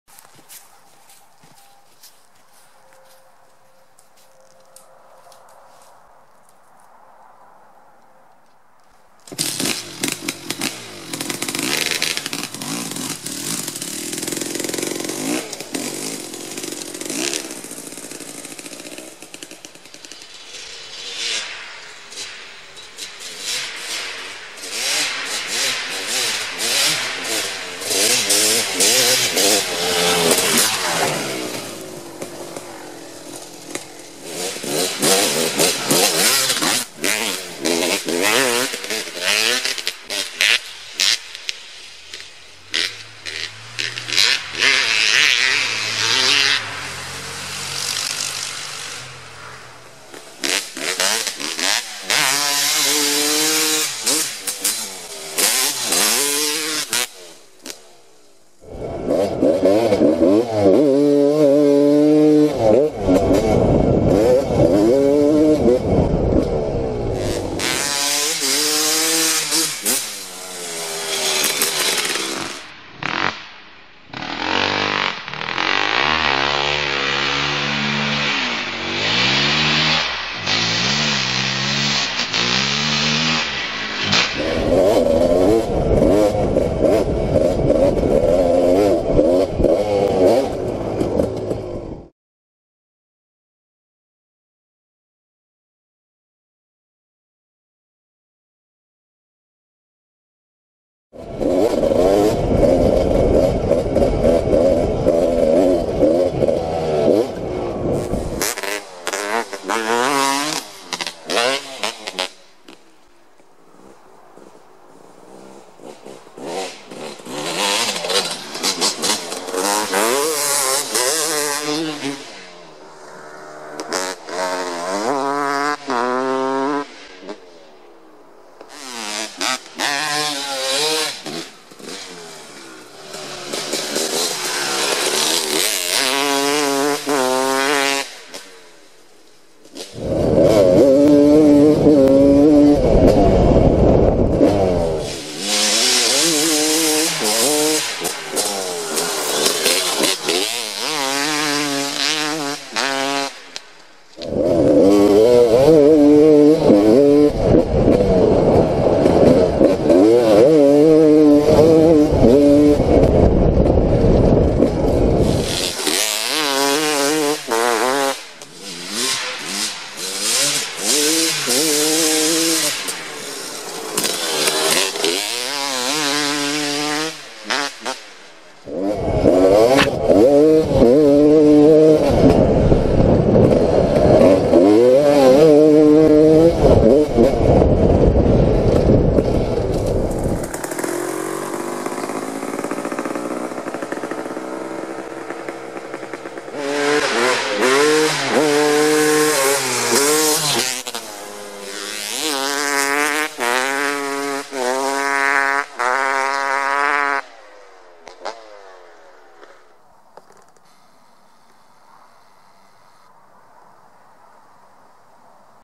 KX250 2-Stroke Braaaps Sounds!
My 2003 Kawasaki KX250 2-stroke. Enjoy that sweet 2-stroke braaap sounds!